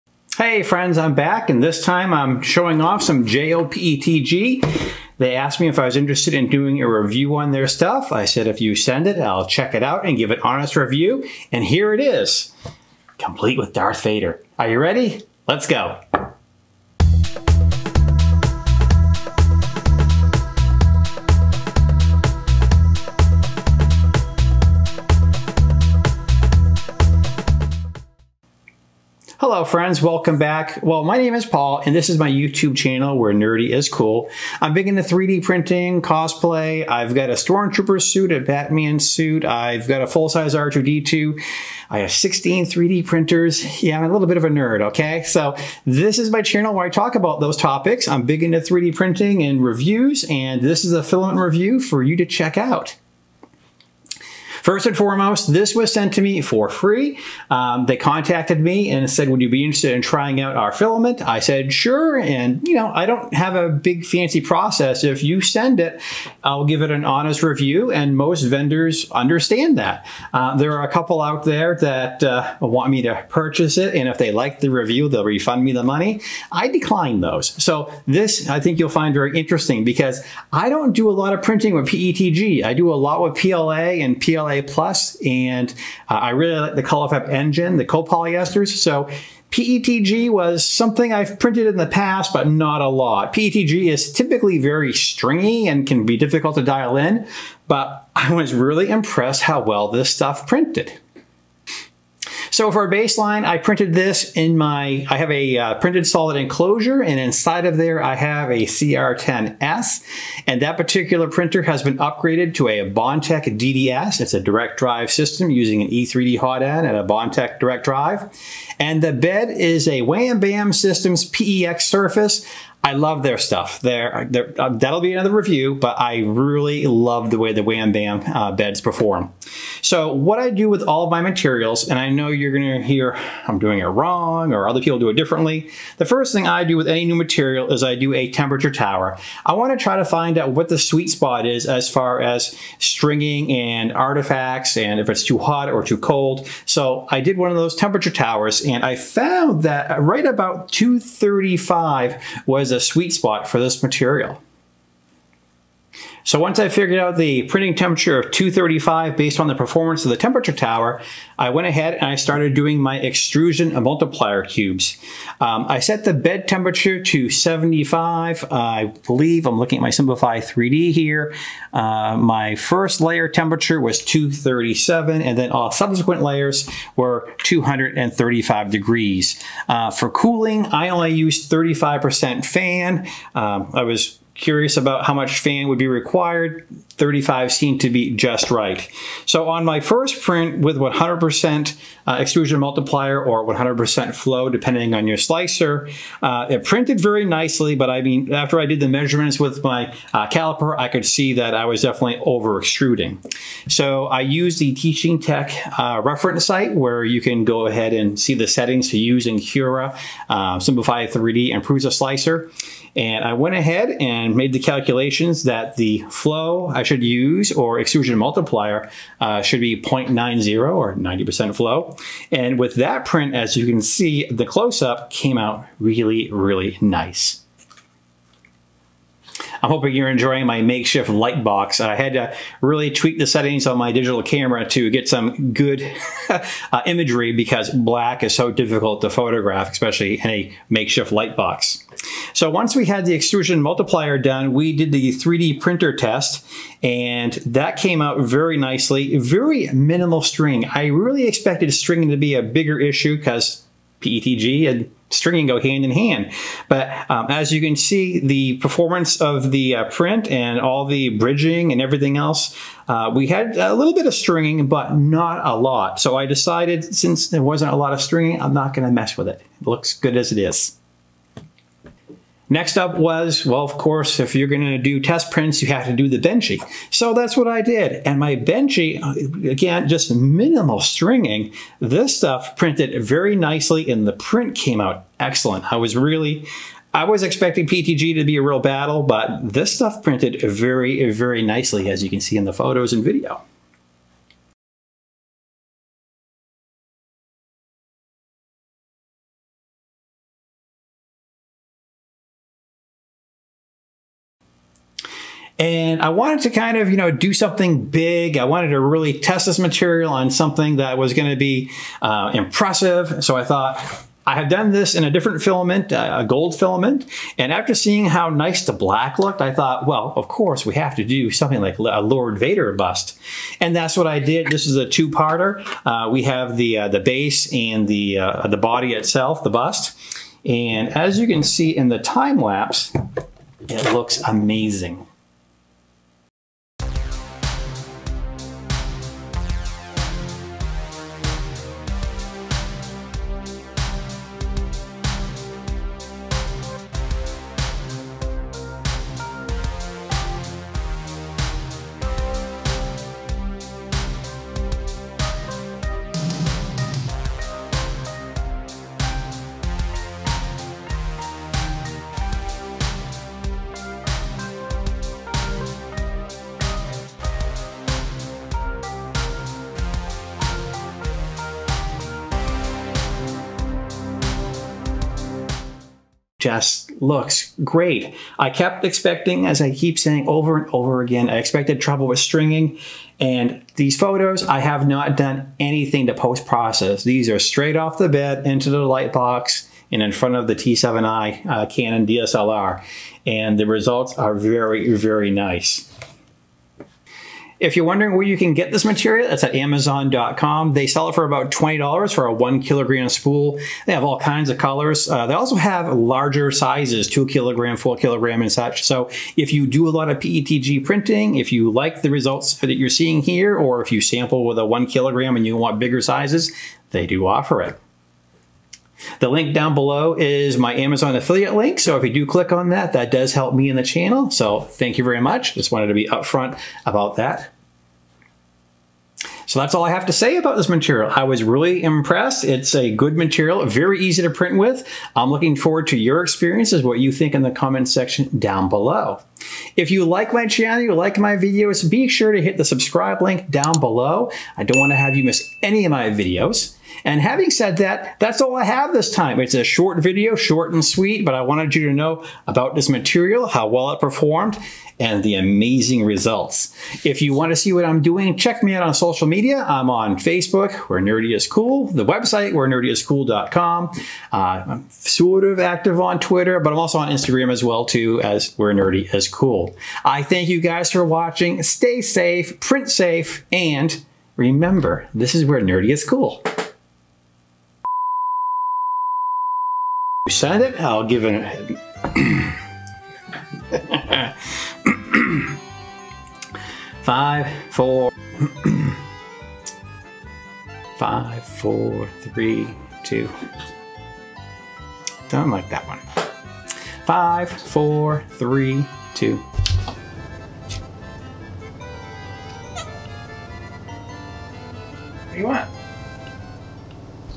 JAYO PETG Filament Review!
JAYO was kind enough to send me a 1kg spool of their Black PETG 3D Printer Filament to evaluate and review.  After doing some test prints to determine the best printing temperature and extrusion/flow rates, I did some prints with it.  I did the usual, popular test models, such as the Benchie and 3D Printer Test.  Then I had to do the amazing Darth Vader bust by Eastman (link below).  I really expected a lot of stringing issues with PETG and I had minimal!  I found this material to be very easy to print with.

The JAYO PETG filament was tested on my Creality CR-10S 3D Printer, upgraded to a Bondtech DDS (direct drive system) and using a Wham Bam Systems PEX bed system.

The printer is in a PrintedSolid Enclosure.

I printed with the first layer at 237C, then 235 for the rest.  Bed temperature was set to 75C.

1 kg spool

2 kg spool

Don't forget our Web Forums!